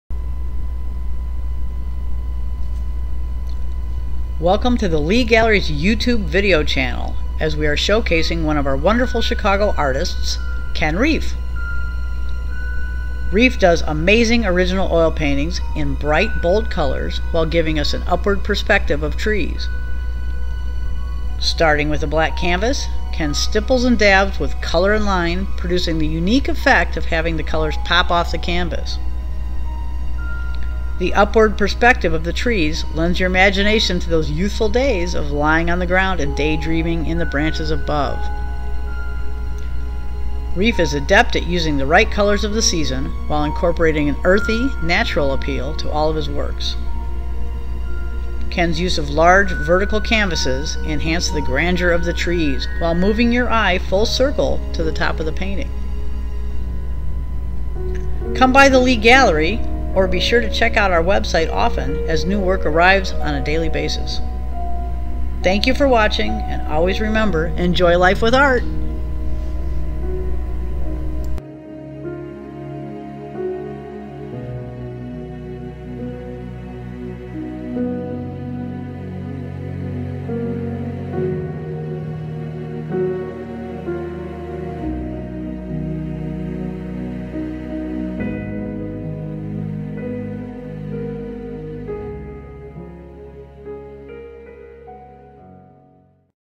The Leigh Gallery Presents Ken Reif
A short description and photo presentation of the works of Chicago artist, Ken Reif.